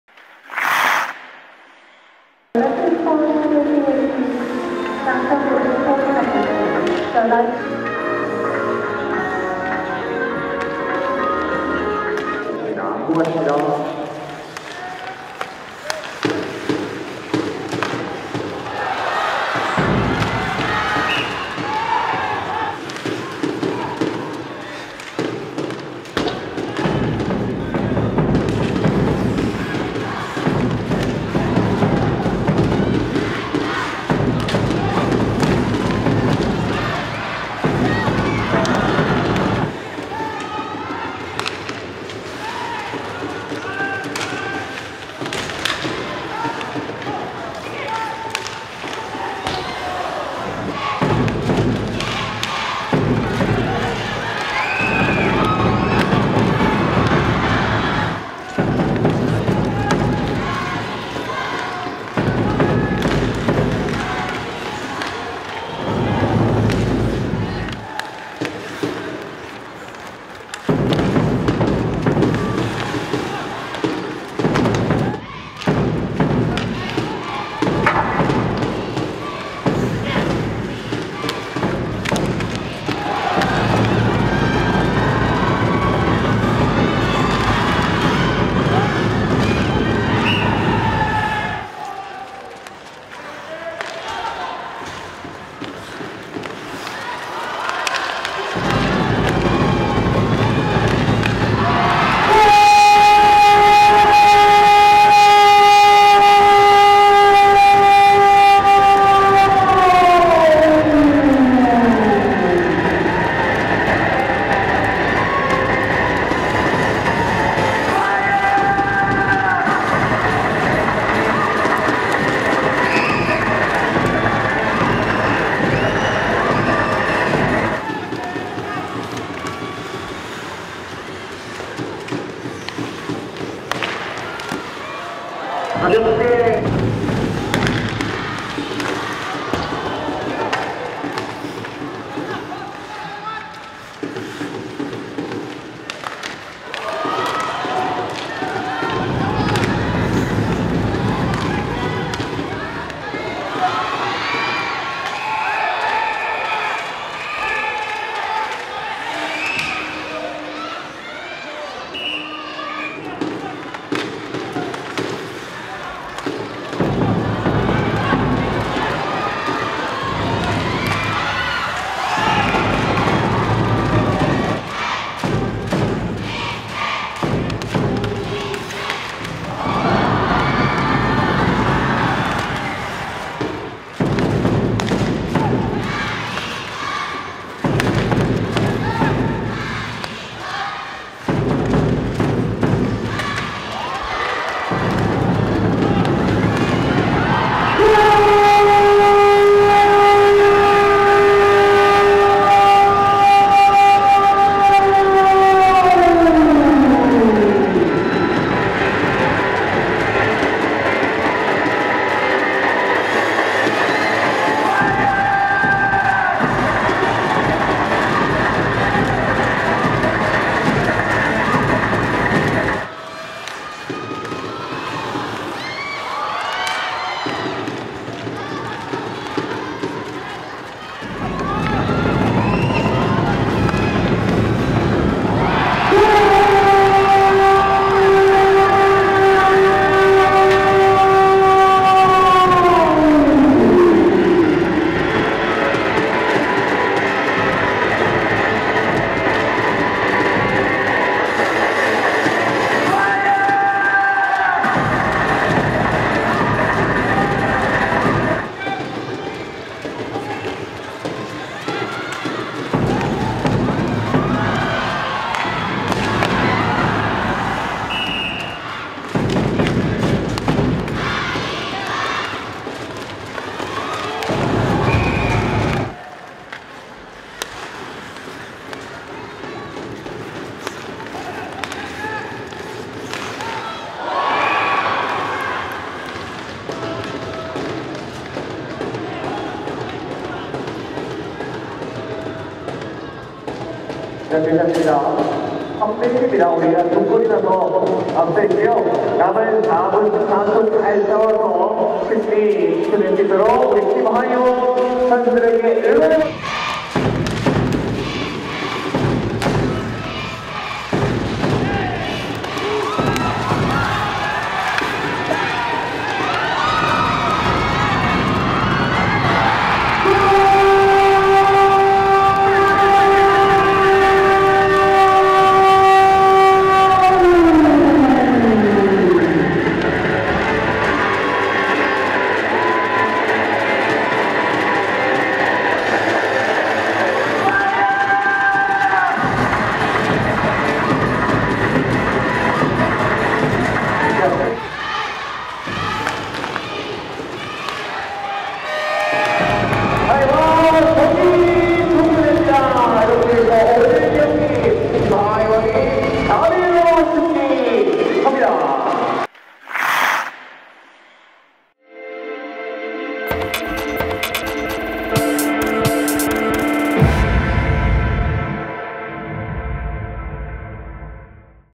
HL | High1 vs Nippon Paper Cranes | 2018. 10. 7
*최종 스코어 표기 때 '2피리어드 종료'라고 잘못 기입되었음을 알려드립니다. '2피리어드 종료'가 아닌 '경기 종료'가 맞습니다.

HIGHLIGHTS
Asia League Ice Hockey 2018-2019
High1 vs Nippon Paper Cranes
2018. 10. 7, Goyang

*영상 구매 및 제작 문의

*온더스포츠의 모든 컨텐츠는 여러분들의 후원을 통해 제작됩니다.*
*영상 제작 후원하기